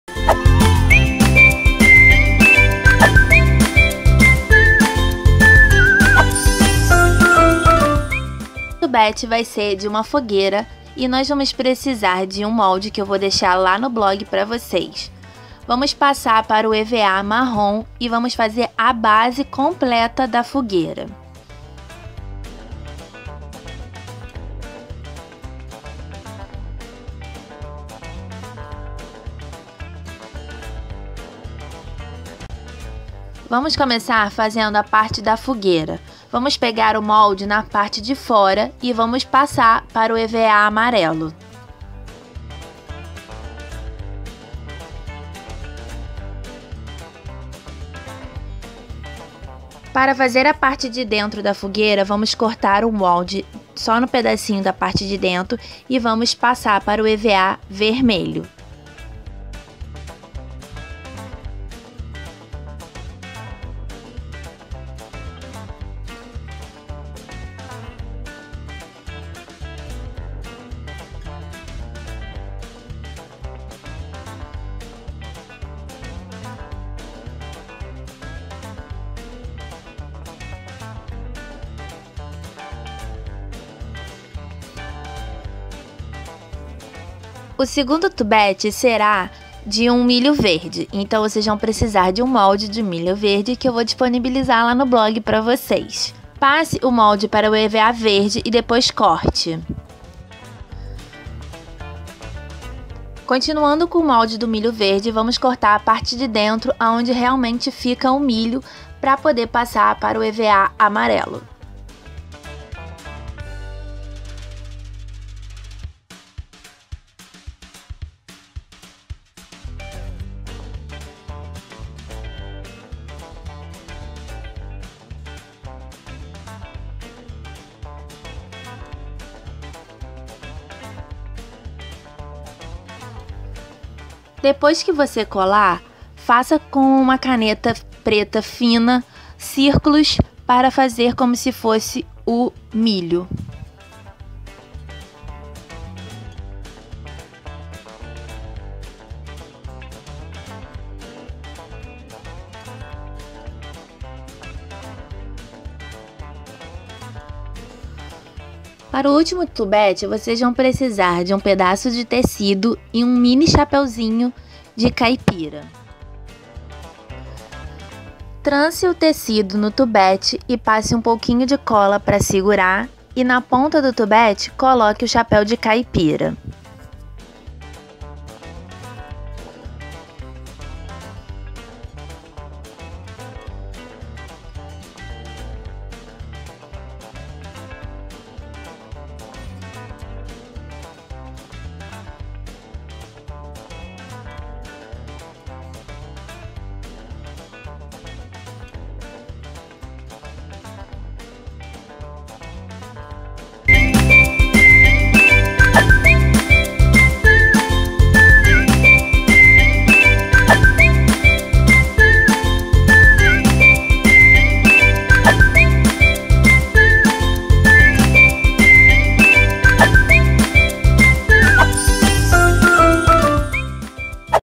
COMO FAZER TUBETE PARA FESTA JUNINA - FAÇA SUA FESTA
Nesse vídeo mostro como fazer tubete para festa junina.

COMO FAZER TUBETE PARA FESTA JUNINA - FAÇA SUA FESTA | POR CAROL GOMES

HOW TO MAKE TUBE FOR FARM PARTY - MAKE YOUR PARTY | BY CAROL GOMES

In this video I show how to make a cartridge for a FARM party.

CÓMO HACER TUBO PARA EL PARTIDO DE LA GRANJA - HAGA SU PARTIDO | POR CAROL GOMES

En este video muestro cómo hacer un cartucho para una fiesta de GRANJA.